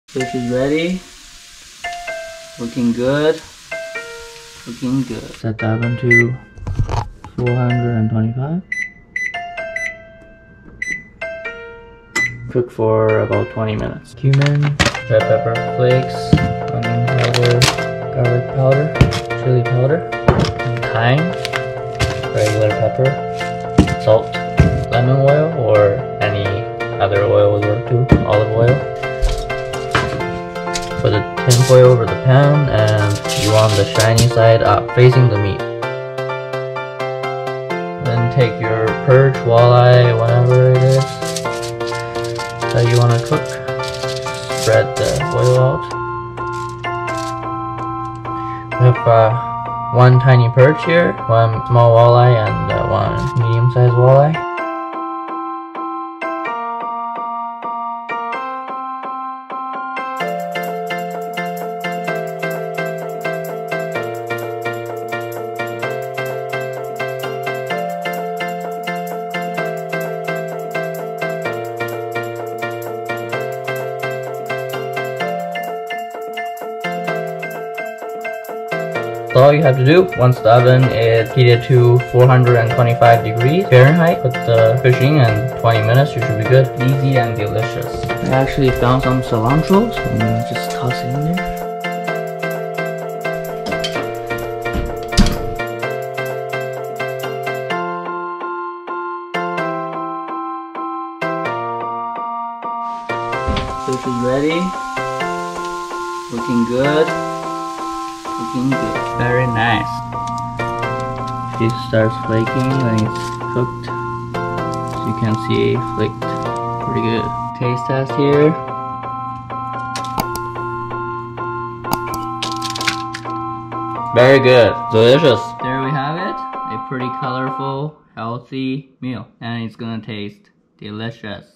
How To Oven Bake Fish (Easy, Healthy & Delicious)
In this video I show you how to oven bake your fish fillets:
- What temperature to set the oven.
- How long to cook the fish.
- What spices to put on the fish.
- How to tell when the fish is cooked.